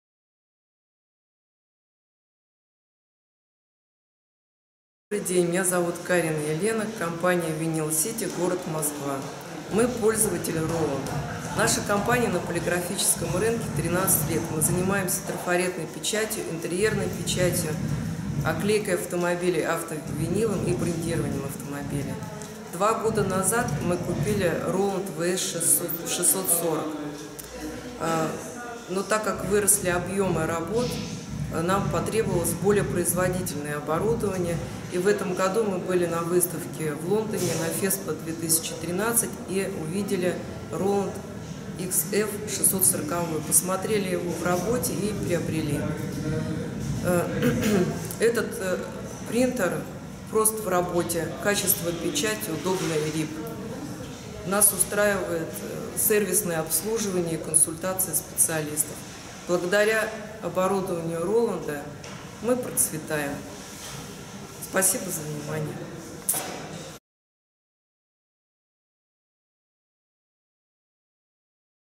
"I'm a Roland User" - Vinyl City, Russia (English sub)
Roland users are a proud bunch and happy to share their Roland experience.